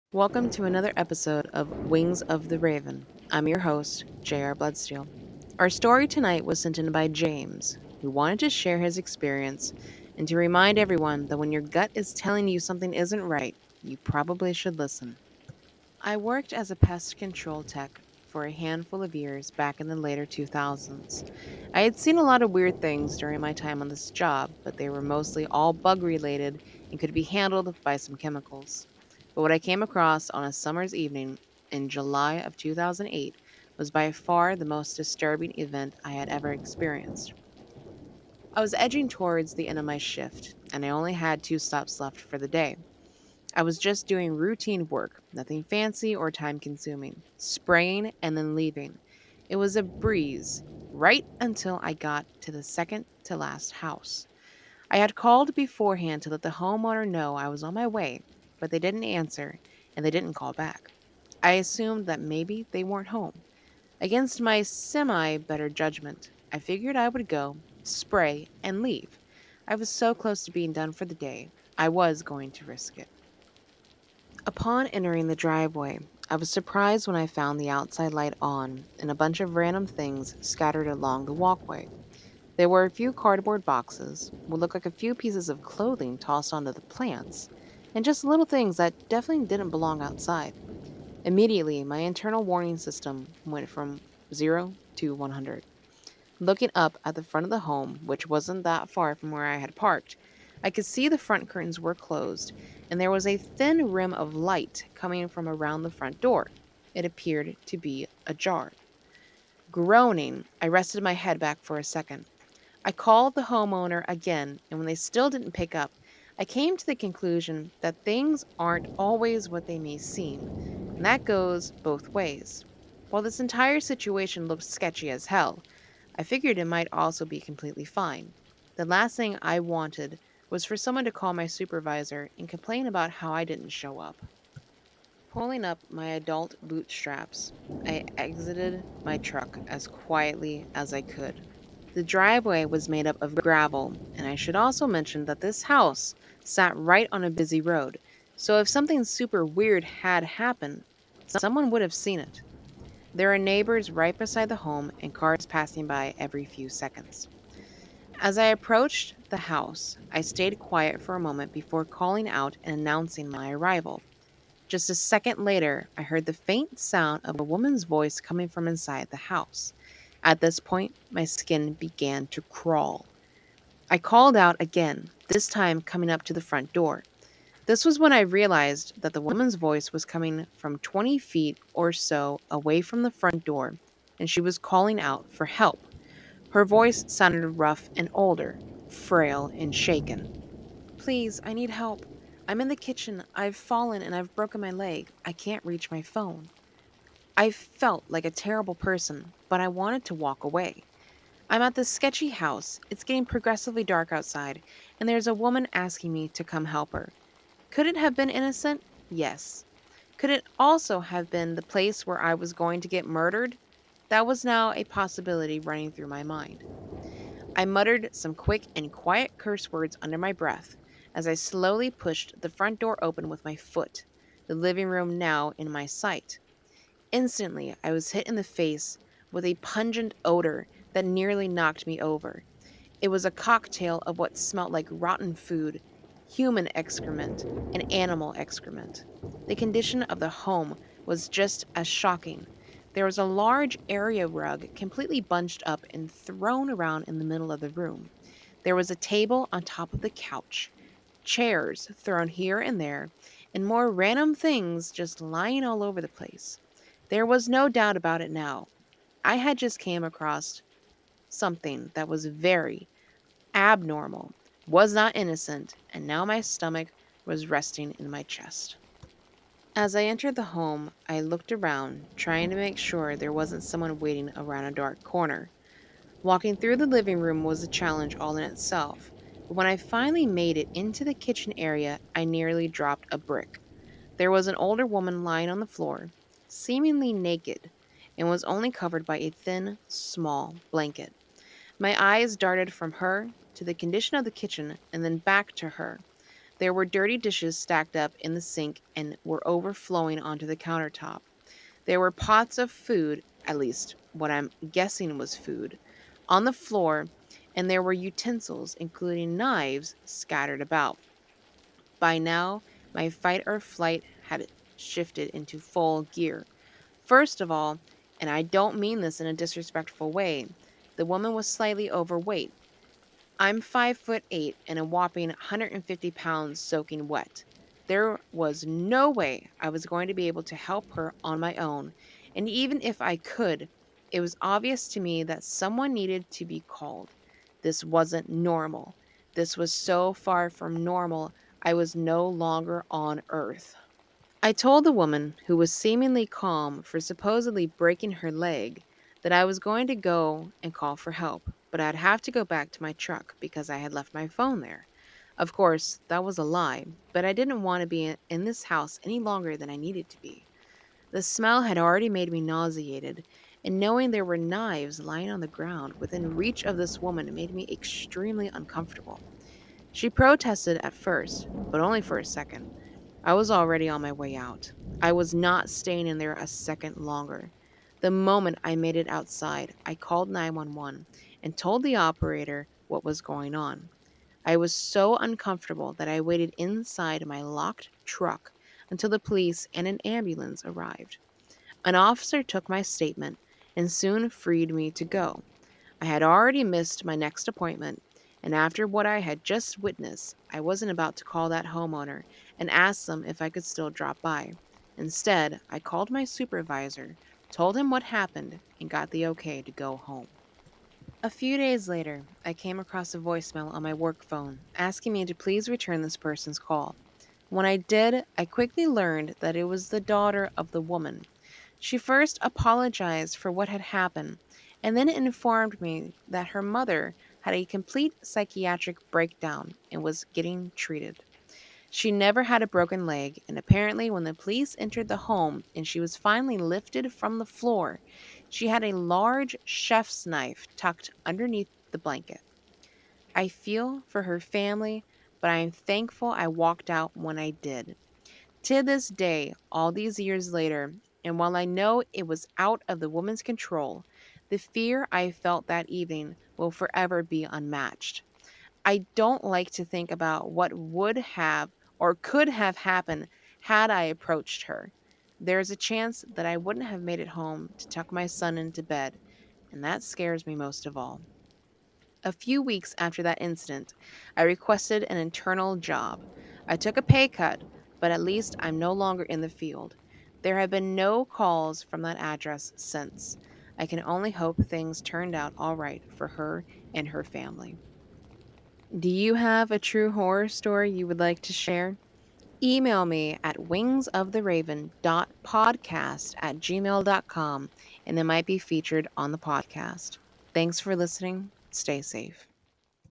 "The Unanswered Call" A True Horror Story
In this episode, I recount a story that was sent in by James.  Sometimes, it's best to take a pay cut than chance fate.